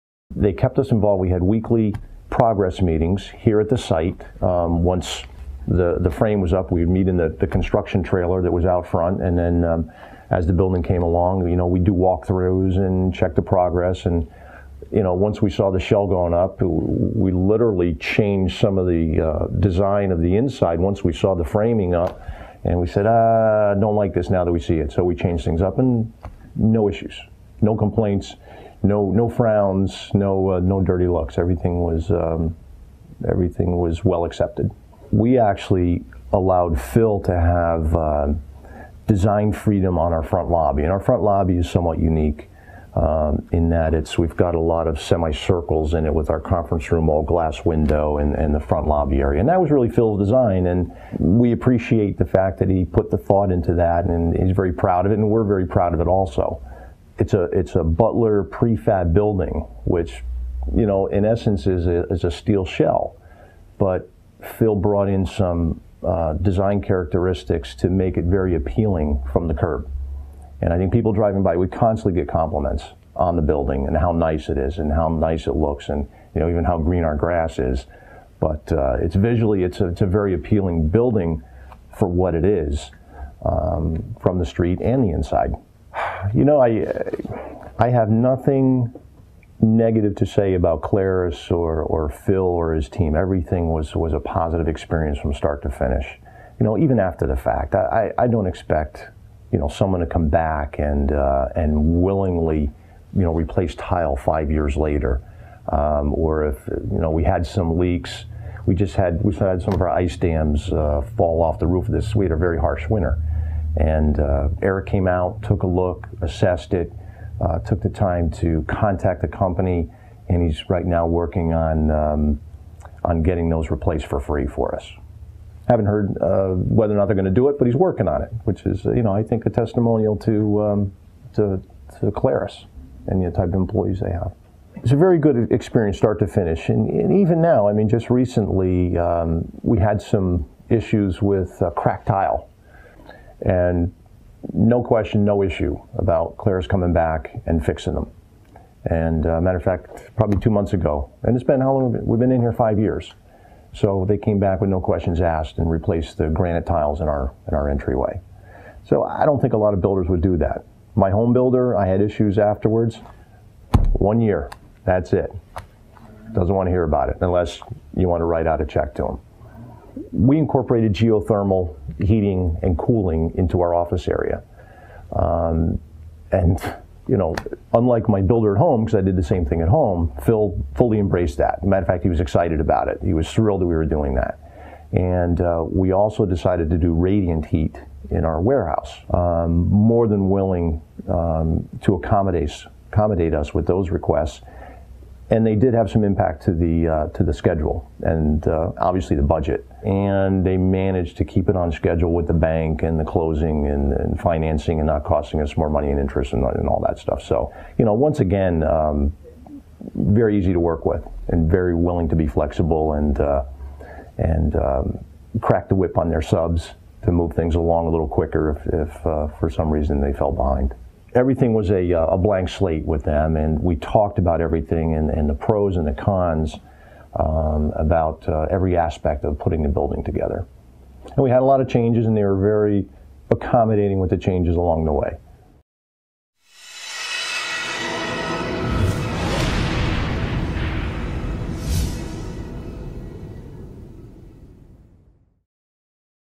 Mark Kuligowski, Creative Mailing
Mark Kuligowski, President of Creative Mailing in Monroe CT, discusses the advantages of using Claris Construction and the "green" features that Claris brought to his project.